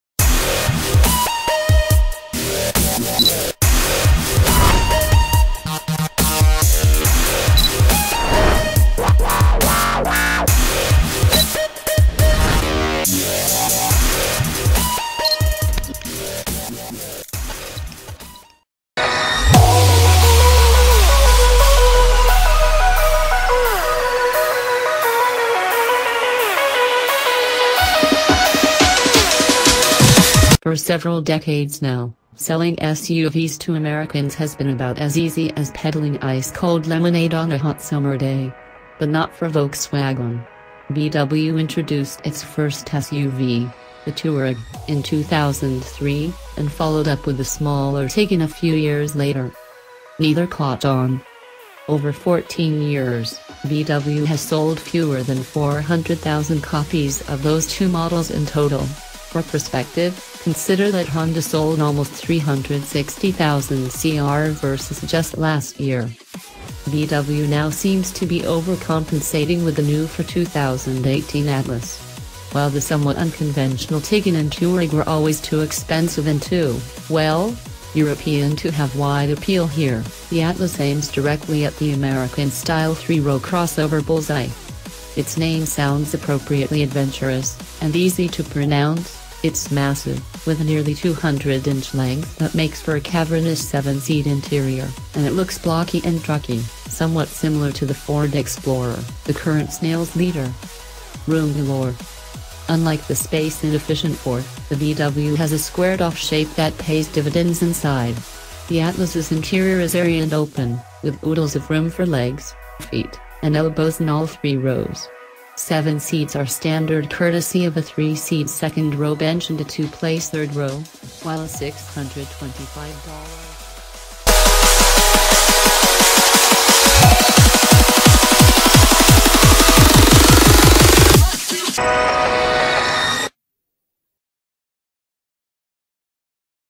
NEWS UPDATE!!!2018 VOLKSWAGEN ATLAS V 6, 8 speed automatic, 3 6L276 hp266 lb ft DOHC
NEWS UPDATE!!!2018 VOLKSWAGEN ATLAS V 6, 8 speed automatic, 3 6L276 hp266 lb ft DOHC - VW now seems to be overcompensating with the new-for-2018 Atlas. While the somewhat unconventional Tiguan and Touareg were always too expensive and too, well, European to have wide appeal here, the Atlas aims directly at the American-style three-row-crossover bull’s-eye. Its name sounds appropriately adventurous (and easy to pronounce); it’s massive, with a nearly 200-inch length that makes for a cavernous seven-seat interior; and it looks blocky and trucky—somewhat similar to the Ford Explorer, the current sales leader.

$50,000

Popular Post !